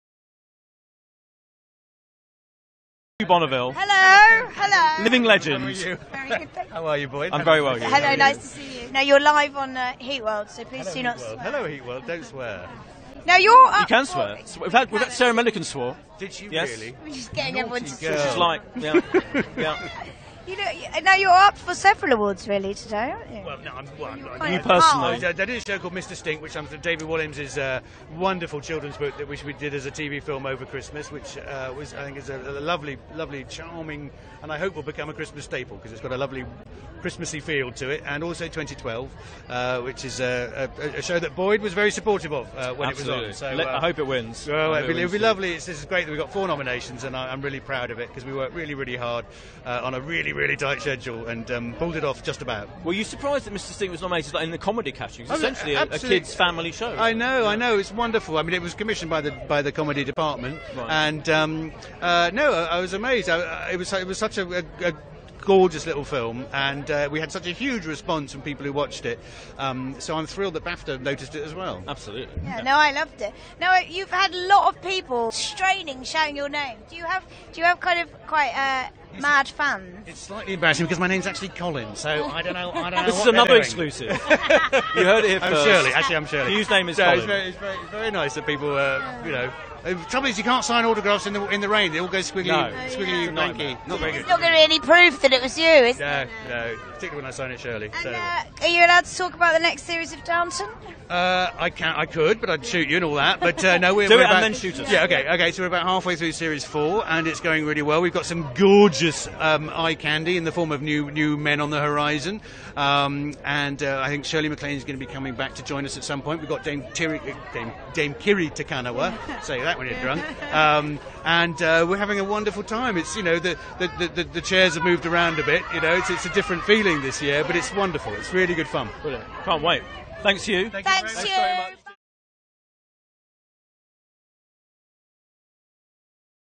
BAFTA TV Awards 2013: Hugh Bonneville talks about his many BAFTA nominations
Hugh Bonneville talks about his BAFTA nominations for Mr Stink and Twenty Twelve at the BAFTA TV Awards.